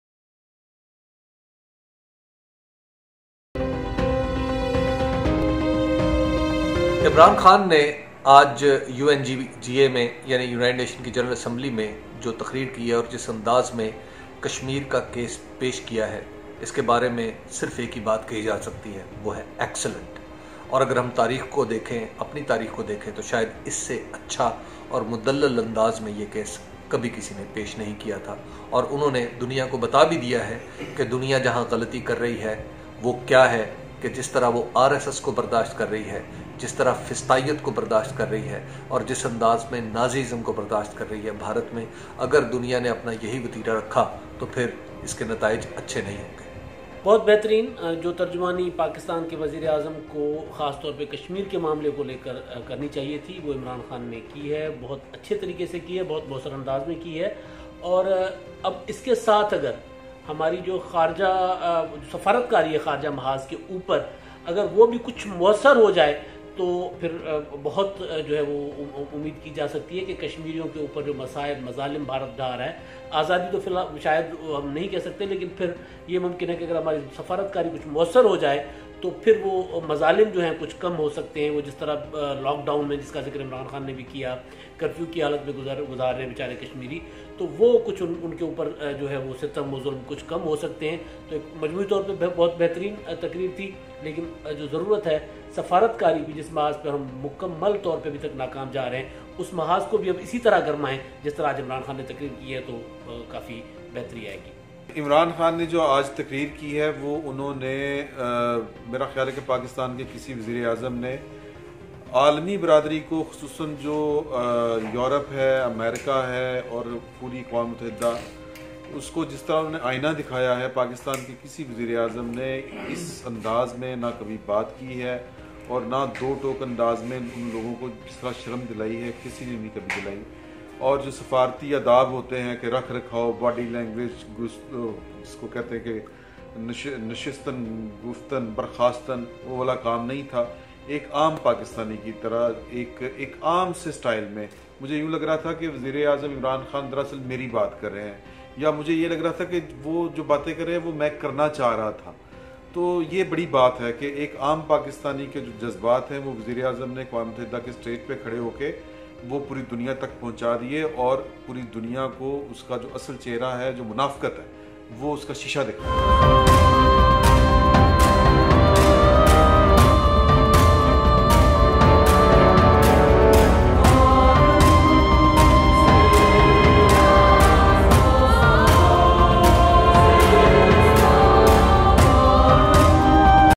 Imran Khan Speech in UNGA | Halaat
Today Imran khan make history in United nation General Assembly Speech